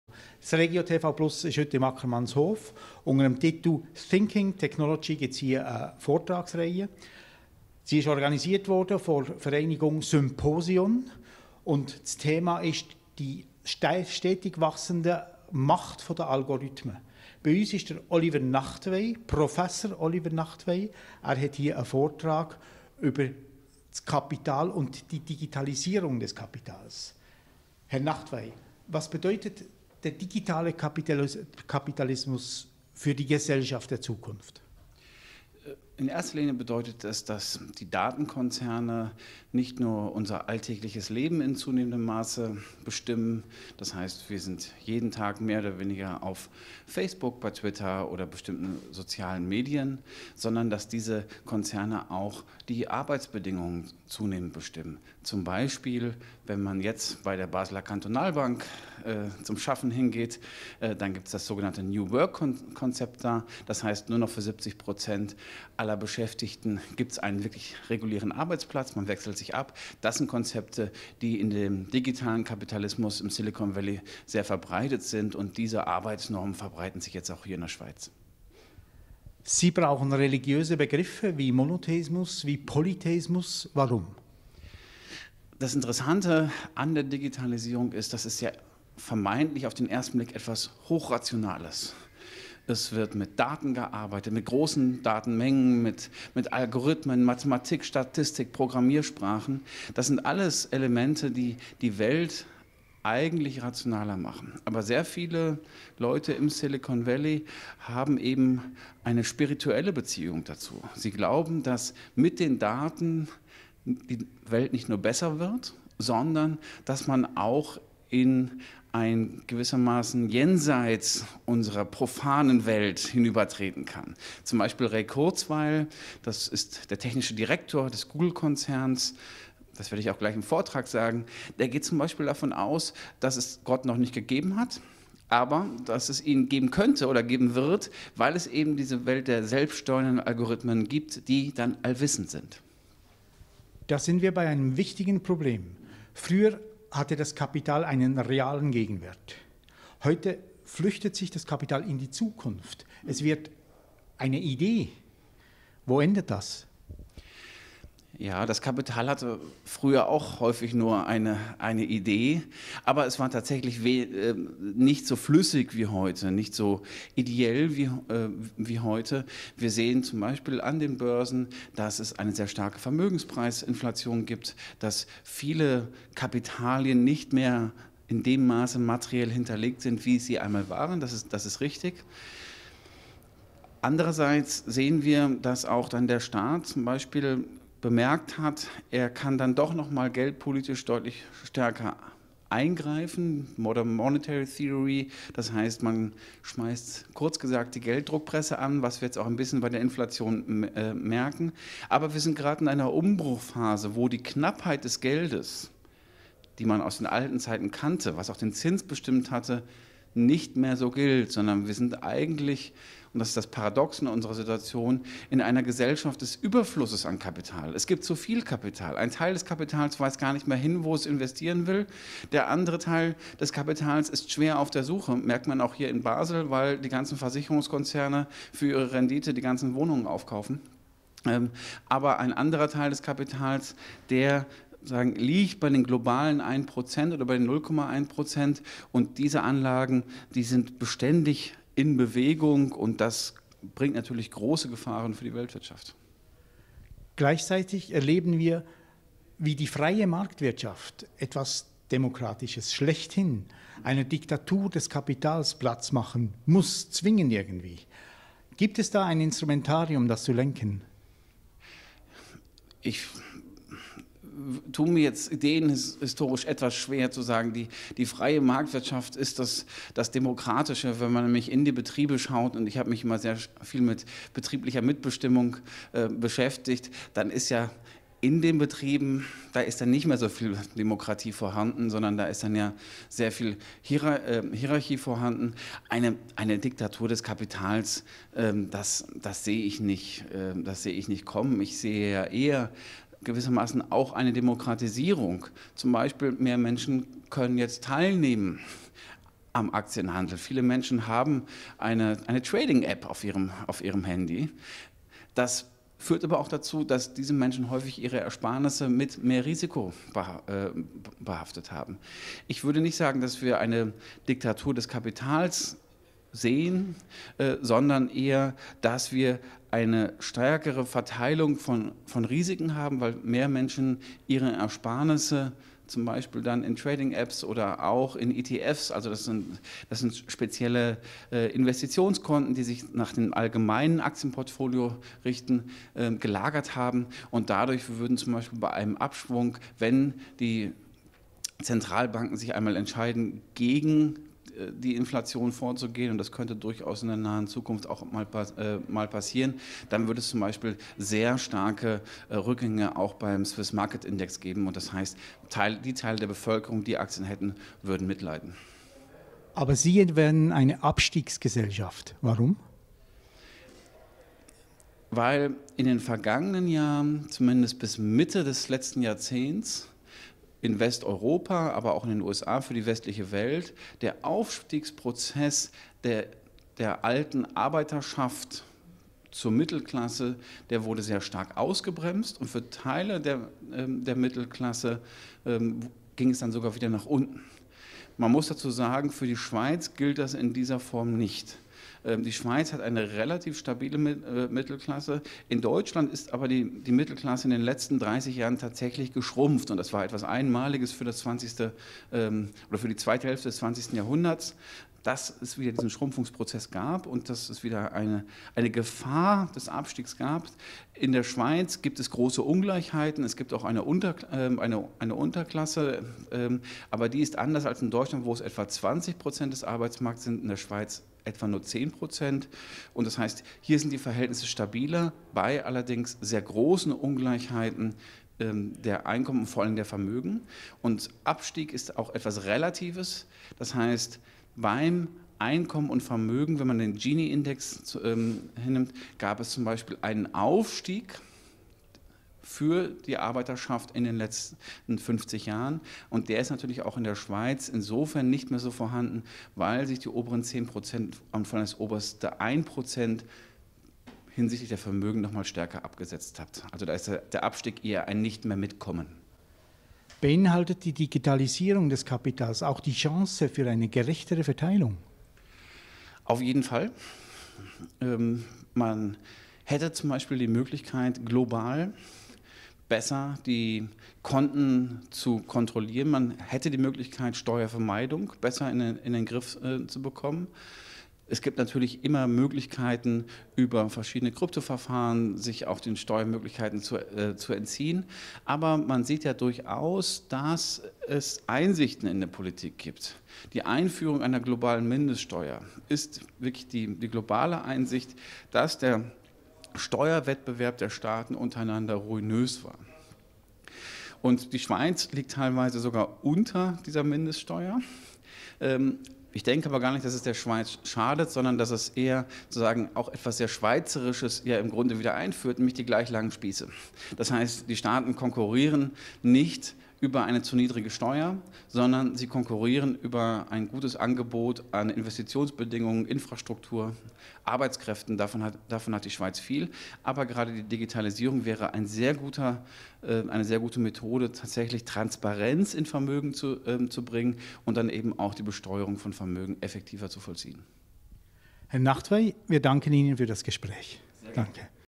Oliver Nachtwey über digitalen Kapitalismus
Ein Treppenhausgespräch im Ackermannshof. Organisiert vom Verein symposion